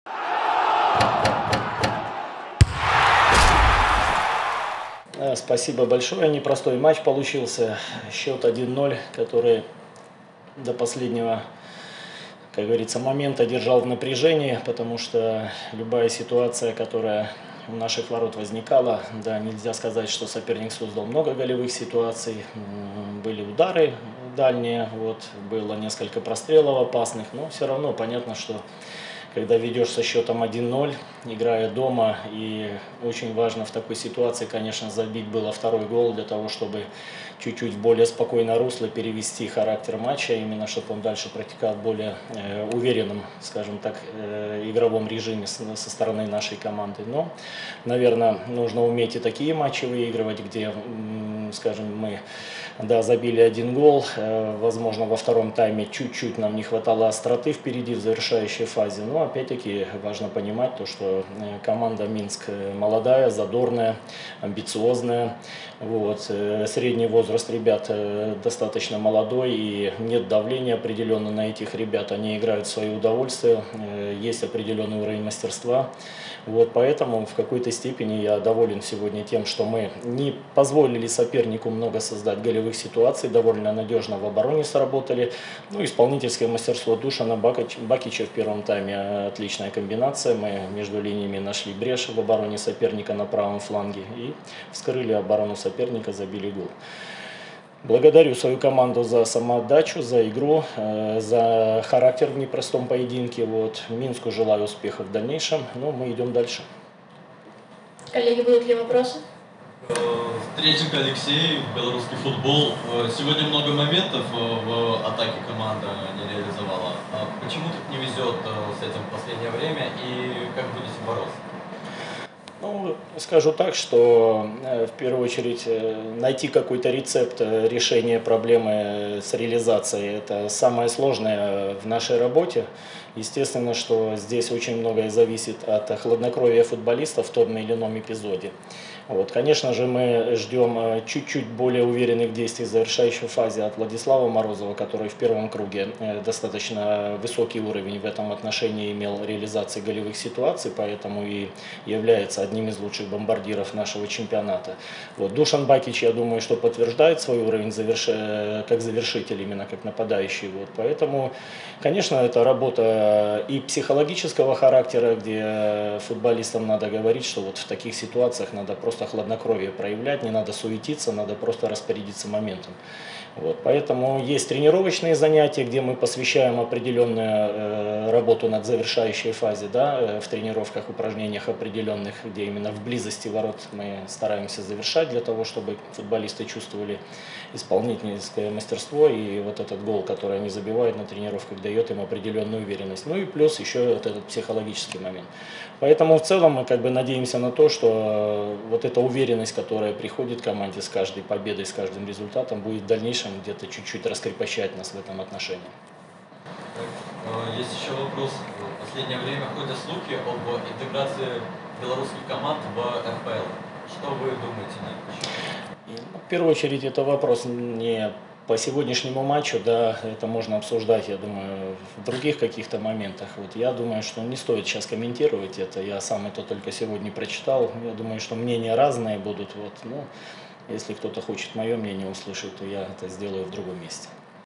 Динамо Минск 1:0 ФК Минск | ПРЕСС-КОНФЕРЕНЦИЯ
0:00 комментарий по игре
2:00 почему не реализовали много моментов?
3:40 белорусские клубы в РПЛ, что думаете?